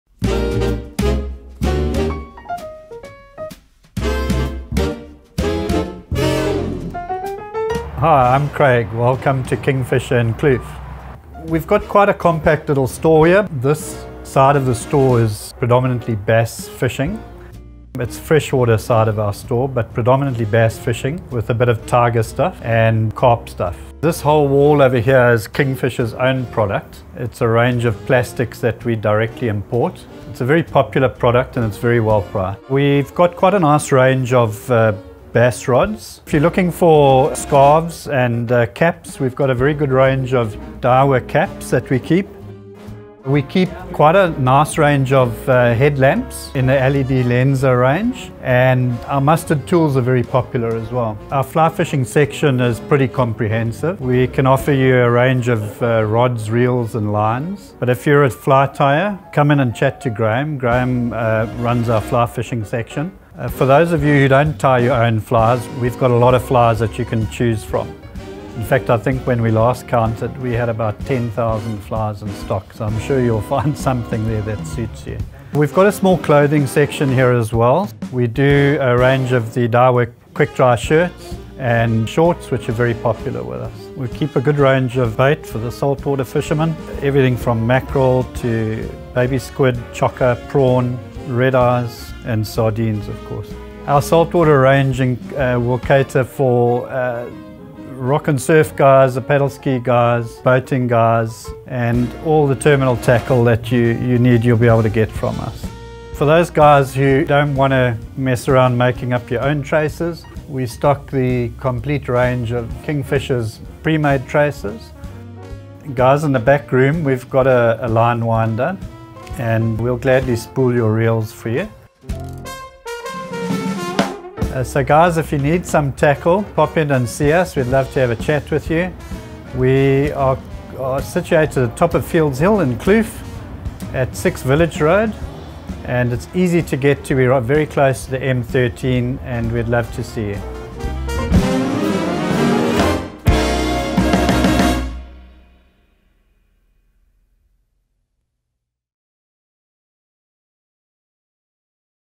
The Kingfisher | Kloof
We would like to introduce you to our Kingfisher Kloof Retail Store. With over "10 000" flies you will find exactly what you're looking for when planning your next trip.

You will find a lot more than just fresh water gear and tackle as we stock a comprehensive range of salt water gear too. From Ski Boaters to Kayakers, Rock & Surf and all the bait to go with it.

Our staff are very friendly and highly knowledgeable, they make a good cup of coffee too.

We are open Monday to Friday 08h00 - 17h00 and Saturday & Sunday 08h00 to 13h00.

You can find our store at:

6 Village Rd, Kloof, Durban, 3610